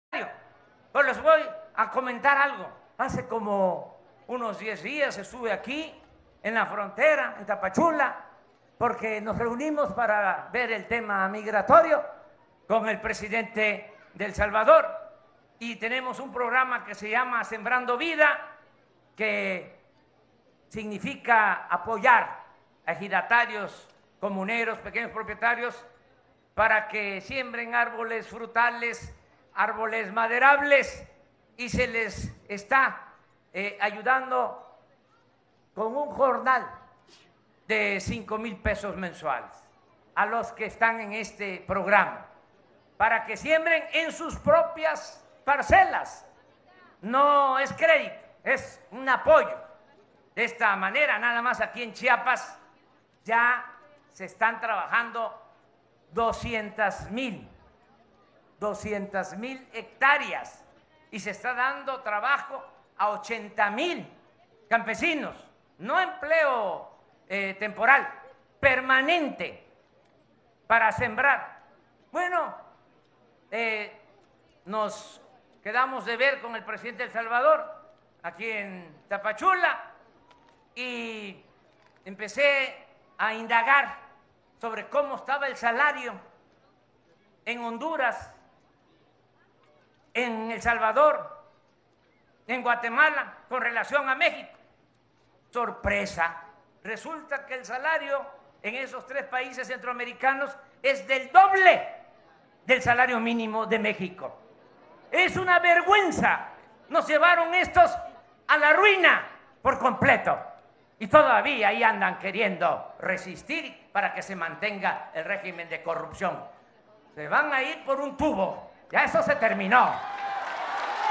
En Chiapas, prometió mejores salarios en el sector salud
AMLO dice que el salario mínimo en México es menor al de Centroamérica: "Es una vergüenza".